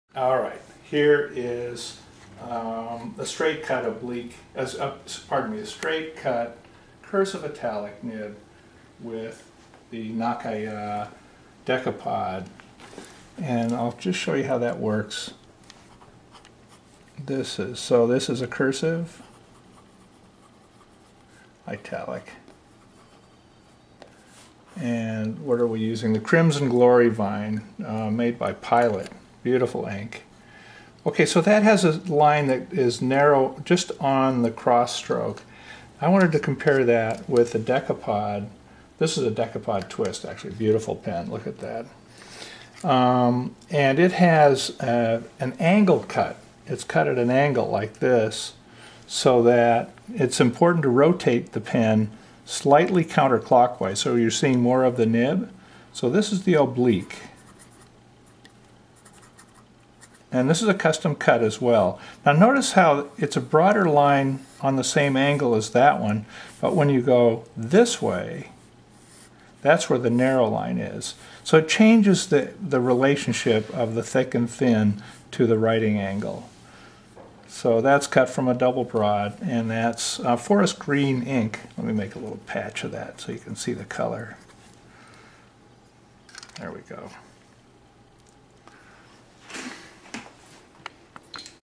Cursive Italic vs. Oblique - customized fountain pen nibs from nibs.com
John Mottishaw demonstrates the difference between the customized Cursive Italic nib and a customized Oblique nib on Nakaya fountain pens using Pilot-Namiki Iroshizuku Crimson Glory Vine and Forest Green bottled ink.